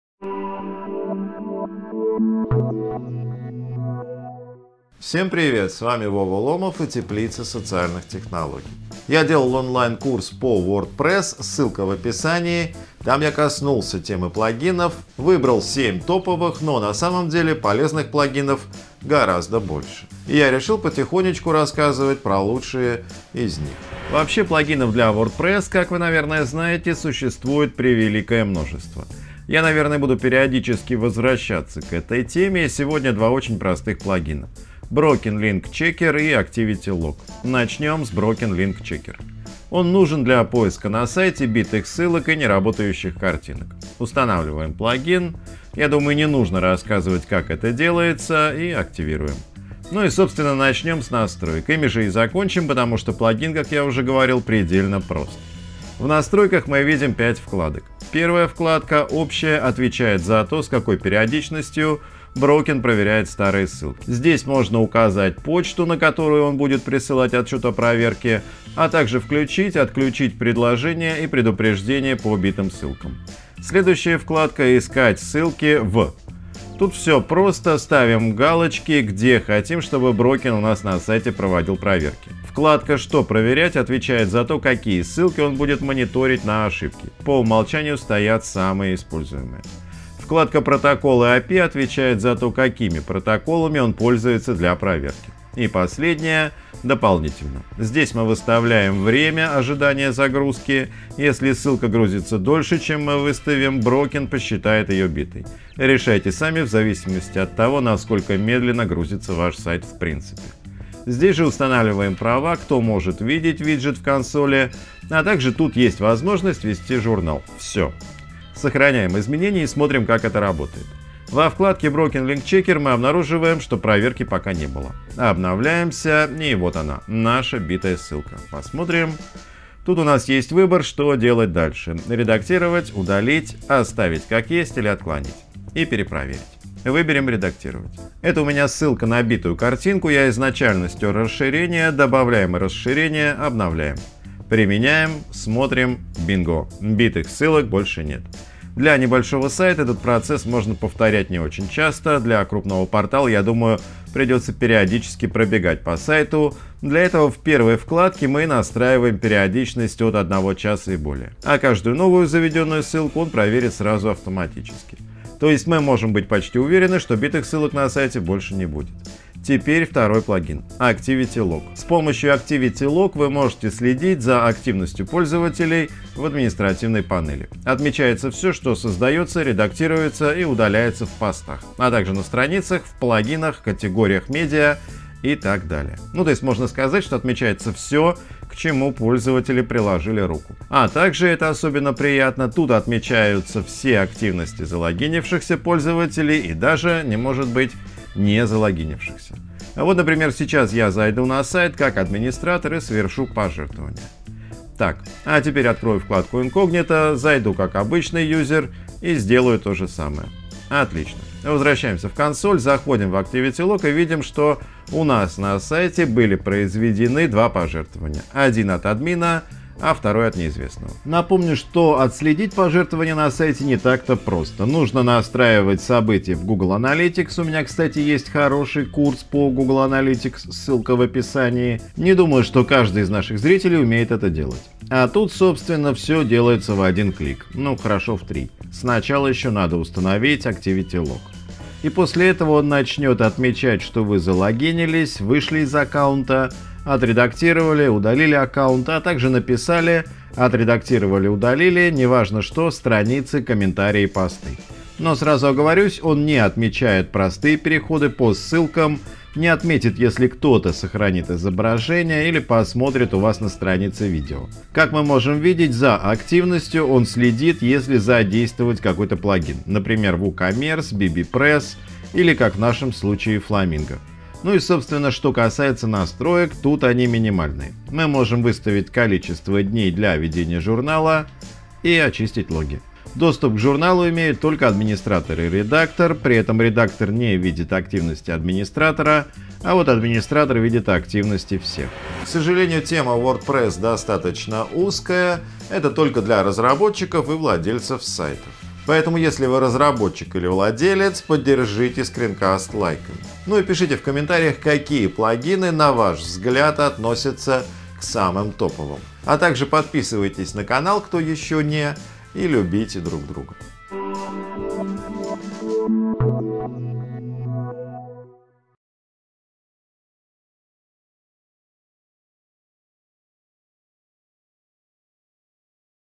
Плагины WordPress #1: Broken link checker - отслеживание битых ссылок, Activity log - активностей
Популярность WordPress базируется на трех китах: простота использования, бесплатно все, что не может быть бесплатным по определению и наличие большого количества сторонних плагинов и тем. Всех достоинств WordPress мы коснулись в курсе по WordPress, коснулись там и темы плагинов, но так как огромное количество их осталось за скобками, мы решили продолжить знакомить вас с самыми популярными и полезными плагинами WordPress.

Сегодня о плагине Broken Link Checker, который проверяет на сайте все ссылки и находит неработающие, а также о Activity Log, который отслеживает активности на сайте залогиневшихся пользователей и частично не залогиневшихся. Например, Activity Log может показывать, кто и в какое время зашел на страницу с плагином для сбора пожертвований и нажал на кнопку пожертвовать.